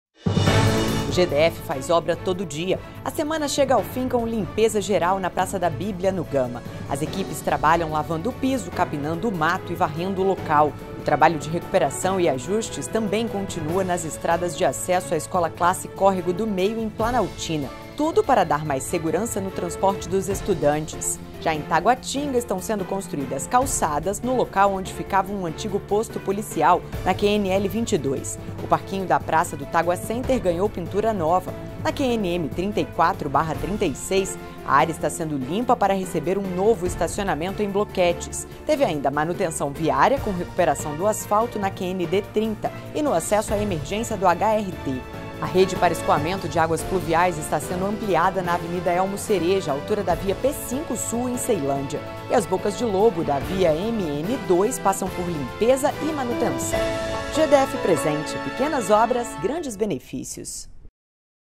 Ações do GDF PRESENTE desta sexta-feira (11/11)
É tempo de ação e as obras no GDF não param! 🚜

O programa GDF Presente veio para dar maior agilidade às ações pela cidade em questões básicas de infraestrutura. Com máquinas espalhadas por todo o DF, são reforçados serviços como tapa-buraco, retirada de entulhos e limpeza de ruas. As ações acontecem todos os dias e as equipes ficam nas cidades até que todas as demandas sejam resolvidas.

✅ Gama - Limpeza geral
✅ Planaltina - Recuperação de estradas de terra
✅ Taguatinga - Construção de calçadas
✅ Taguatinga - Pintura de parquinho
✅ Taguatinga - Construção de estacionamento
✅ Taguatinga - Recuperação de pistas
✅ Ceilândia - Ampliação da rede pluvial
✅ Ceilândia - Manutenção de bocas de lobo

Fique por dentro de tudo o que acontece no DF. 🤳
Acesse o site da Agência Brasília: